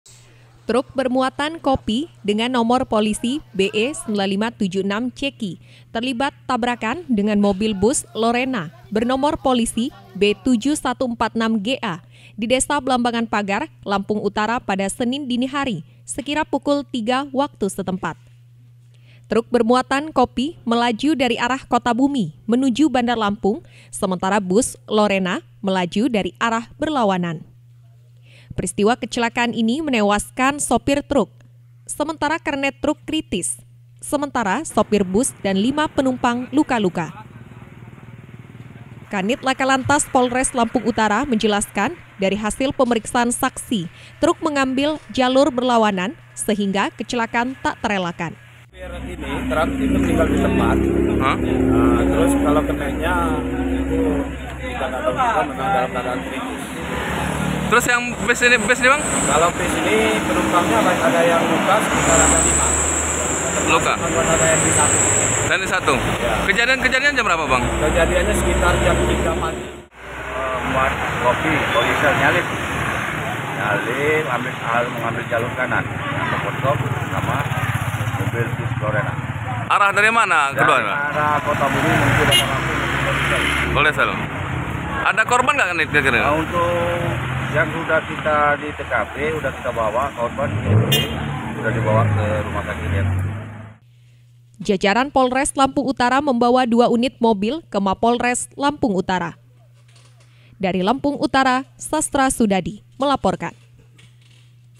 Truk Vs Bus Lorena, 1 Tewas, 1 Kritis Dan 5 Luka
Insiden Kecelakaan Kembali Terjadi Di Blambangan Pagar Lampung Utara Kali Ini Kecelakaan Melibatkan Mobil Truk Pengangkut Kopi Dan Bus Penumpang Lintas Provinsi Akibat Kecelakaan Sopir Truk Meninggal Dunia Satu Kritis Dan Lima Korban Mengalami Luka-Luka